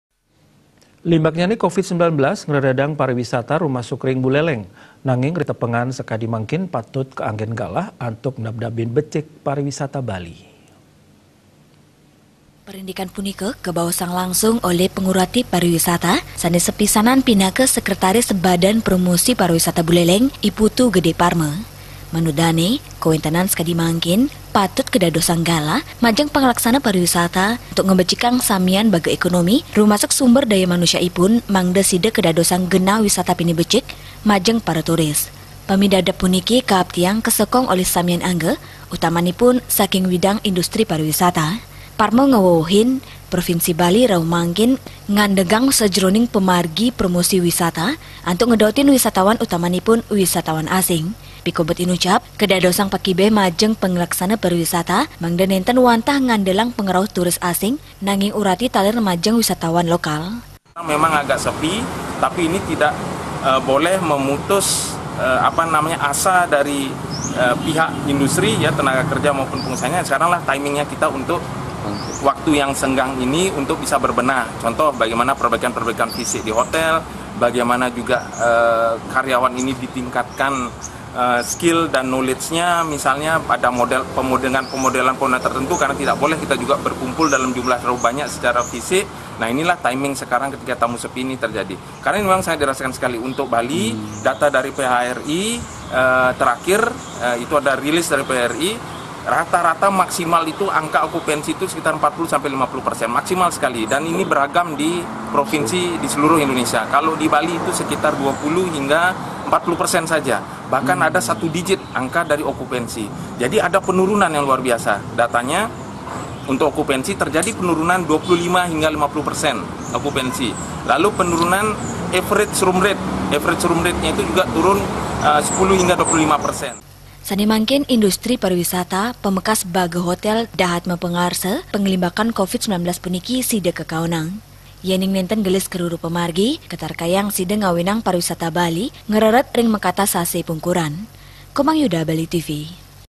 PAMIDABDAB ANTUK WIDANG PARIWISATA
LIMBAKNYANE COVID-19,NGREREDANG PARIWISATA RUMASUK RING BULELENG.NANGING, RITEPENGAN SEKADI MANGKIN, PATUT KAANGGEN GALAH, ANTUK NABDABIN BECIK PARIWISATA BALI.